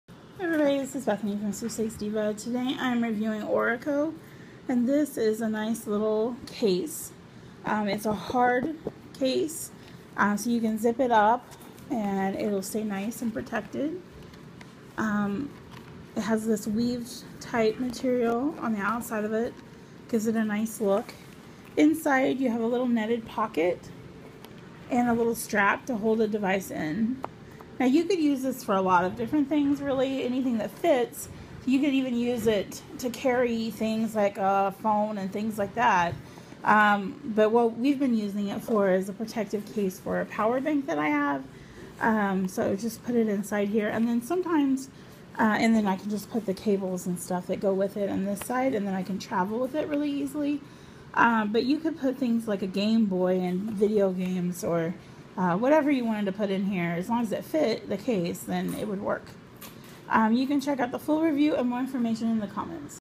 Orico Protective Case for Power Bank
I just got this great case from Orico. This is a great hard case that has a zipper. You can use this to carry a power bank. On the inside you have a stretchy strap to secure your power bank (or whatever device you are using it for). On the other side is a netted pocket that can be use to store cables, adapters, etc. I use mine for my small travel power bank. I like that I can keep the power bank and all the attachments, cable, and adapters in one place. It's small so I just throw it in my bag and take it everywhere with me. Great way to store your items and keep them safe from damage. You could also use this for other items. My kids steal it sometimes for their gameboys. Works great! I received this item complimentary or at a reduced price for my honest review. However, I only recommend products I truly love and use to my readers.